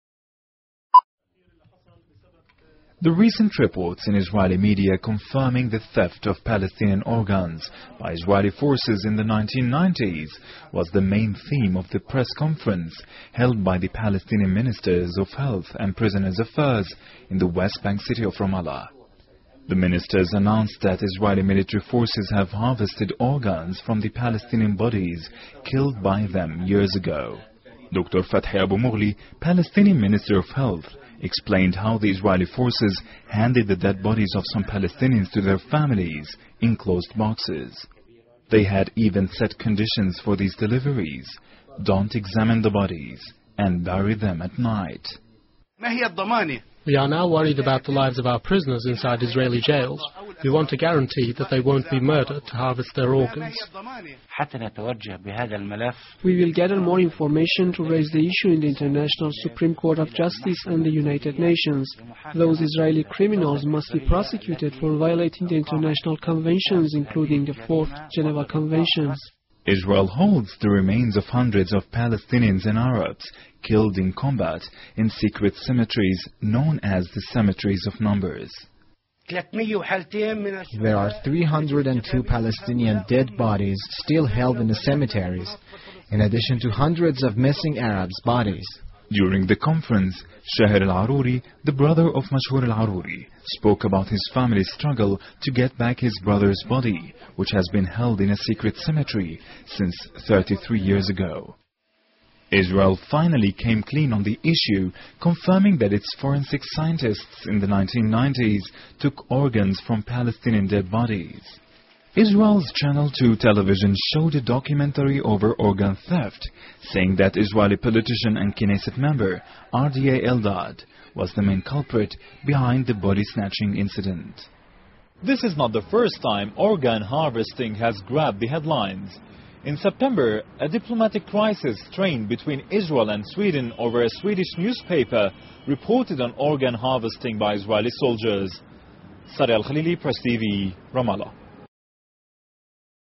Palestinians' organ theft
Israel Comes Clean on Palestinians' Organ Theft

Parliamentarian provides evidence that Israel stripped body organs off Palestinians

Butchers: the hidden truth about Israel's kidney theft ring

How U.S. Rabbis & Israel traffic in human organs & money laundering

Argentine Jewry's dark secret, Los Rufianos de Argentina. Young virgins were broken in and raped, beaten, starved and locked in cages